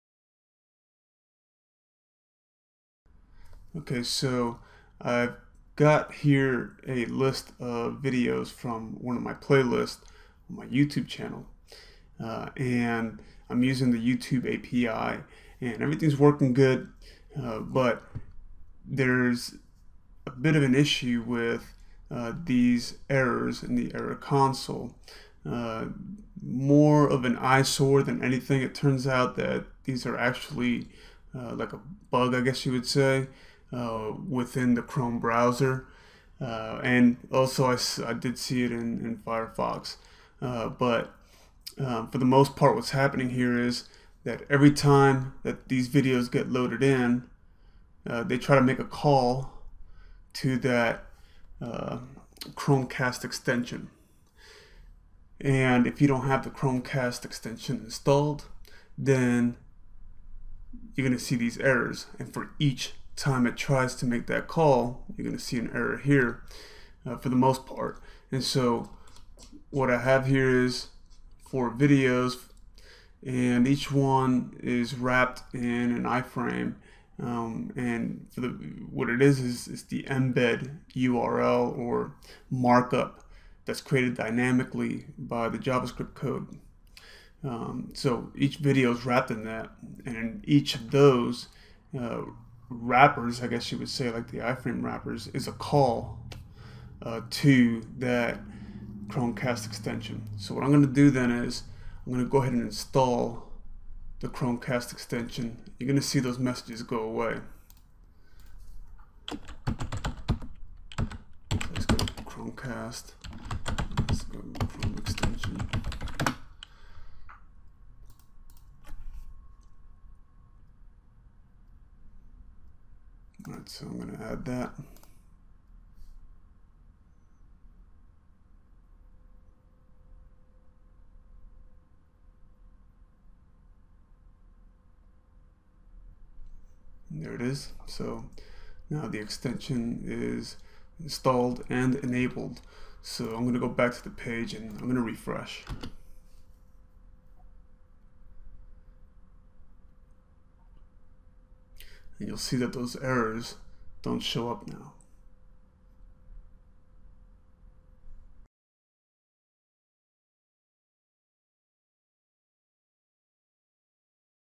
Javascript - Youtube REST Api 1.1 How To Resolve Cast Sender JS net::ERR_FAILED Console Error
INSTALL THE GOOGLE CAST CHROME EXTENSION, THOSE ERROR MESSAGES GO AWAY